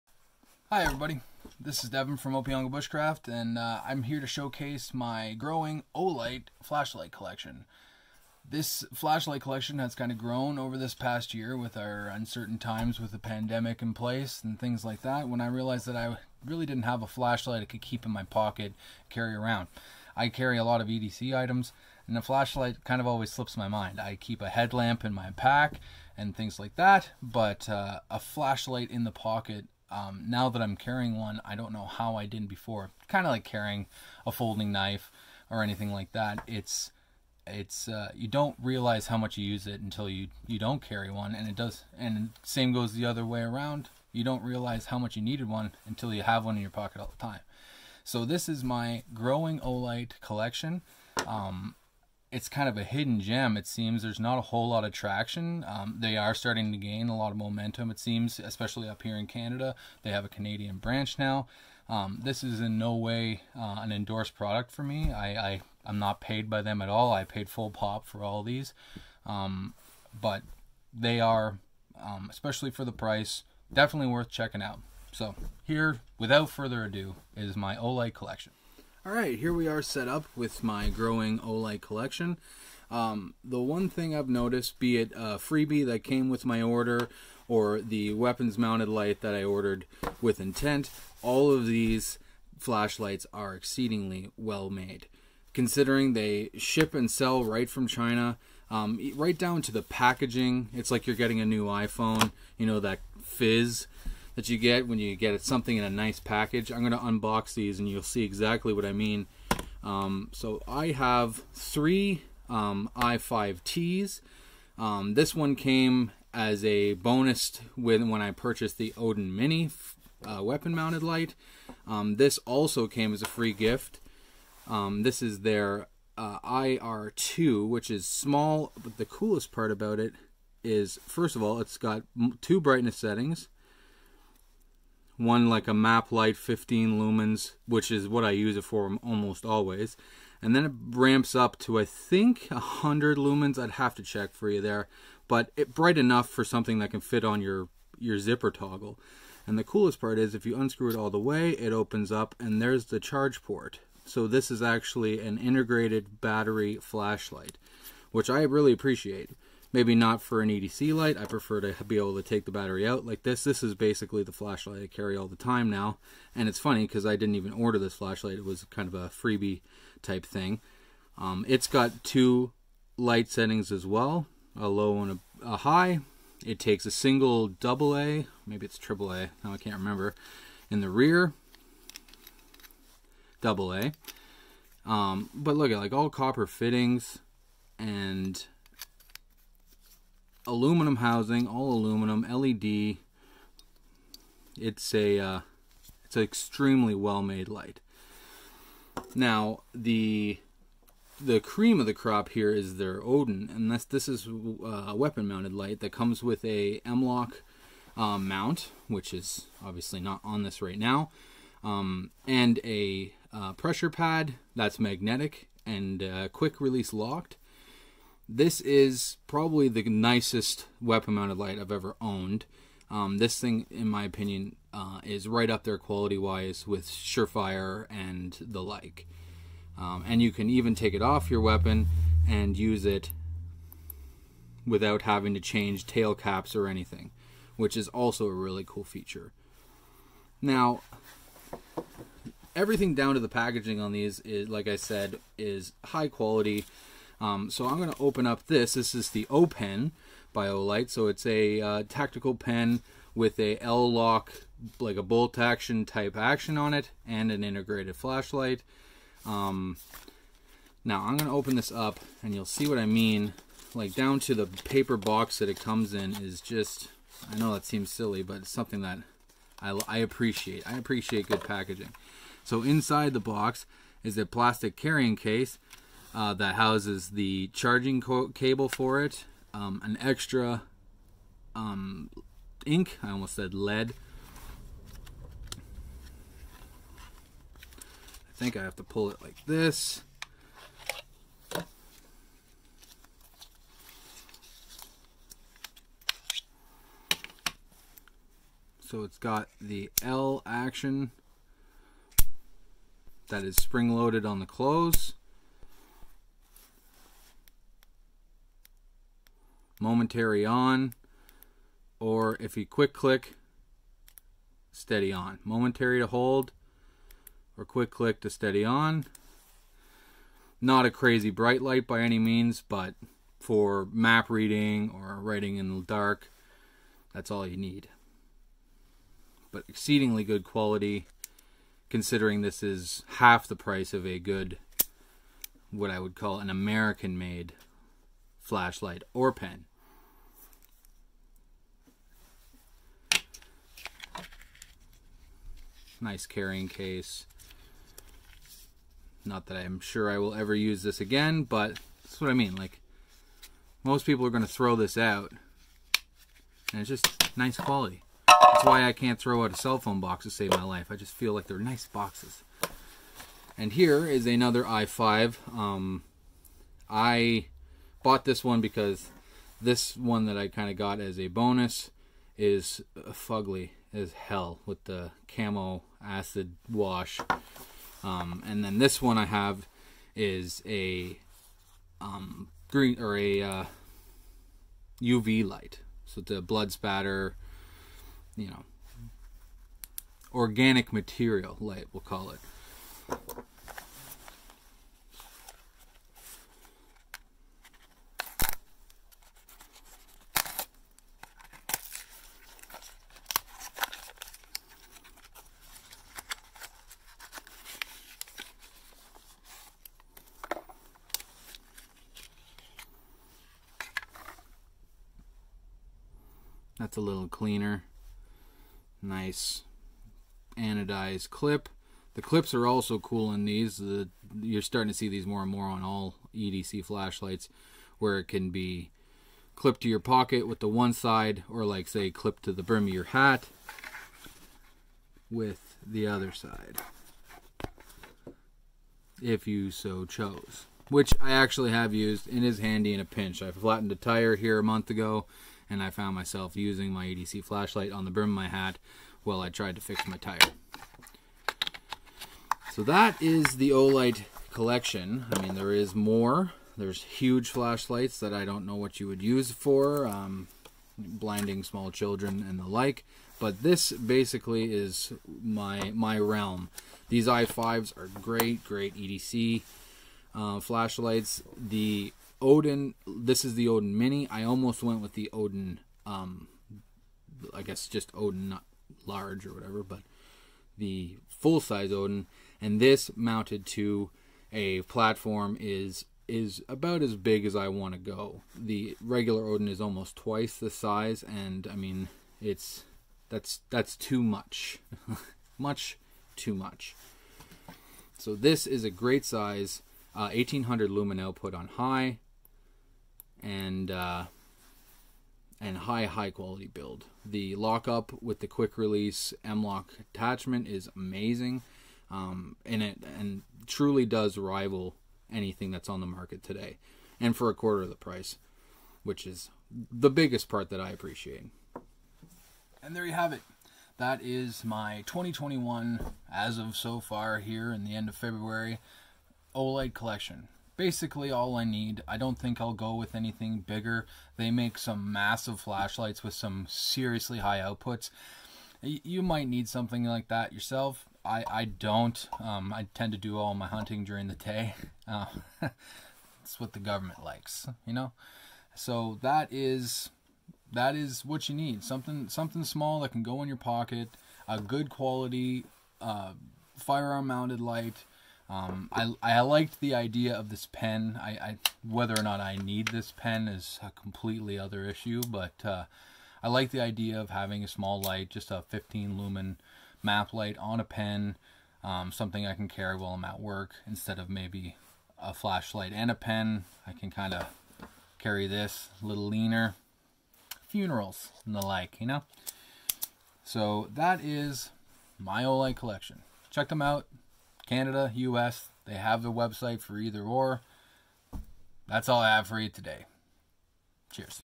My O-Light Flashlight Addiction
Devin's debut gear overview video shows off some of Olight's products!

The O-pen has 4 brightness settings I figured out after the camera was off! so that's cool.